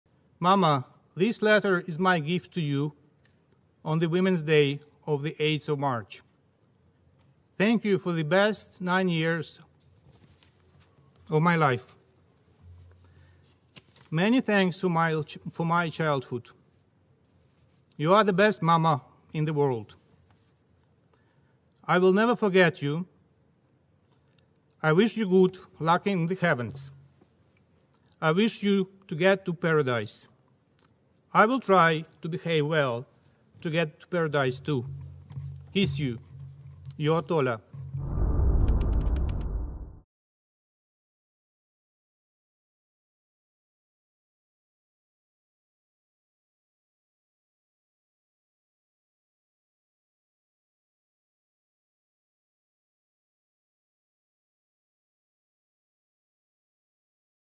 La emotiva carta de un niño de 9 años a su madre muerta en Ucrania: "Gracias por mi infancia"
El embajador de Ucrania ante la ONU, Sergiy Kyslytsya, aprovechó la reunión del Consejo de Seguridad de la ONU de este lunes para leer en voz alta la carta de un niño ucraniano escrita a su madre muerta cuando intentaba huir de la guerra.

Según explicó el embajador es la carta que un niño de 9 años le escribió a su madre con motivo del Día de la Mujer el pasado 8 de marzo. "Mamá, esta carta es mi regalo", comienza la carta, "en el Dia de la Mujer". "Gracias por los mejores 9 años de mi vida, muchas gracias por mi infancia. Eres la mejor madre del mundo", continúa la carta leída por Kyslytsya.